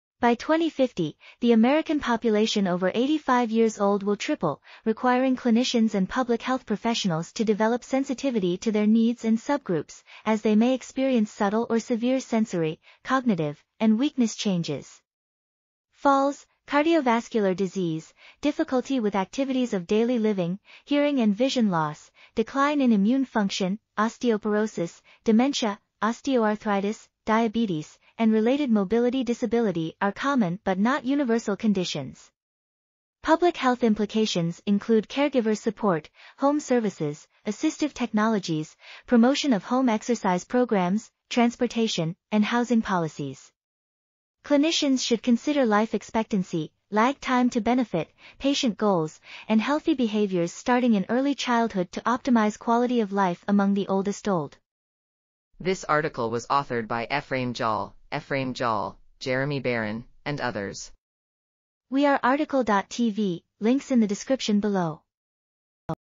Age-Related Diseases and Clinical and Public Health Implications for the 85 Years Old... | RTCL.TV
### Keywords ###

### Article Attribution ###
Title: Age-Related Diseases and Clinical and Public Health Implications for the 85 Years Old and Over Population
Authors: Efraim Jaul, Efraim Jaul, Jeremy Barron ,and Jeremy Barron
Publisher: Frontiers Media S.A.

### Video Timestamps ###
0:00:00 - Summary
0:00:55 - Title
0:01:01 - Tail
0:01:05 - End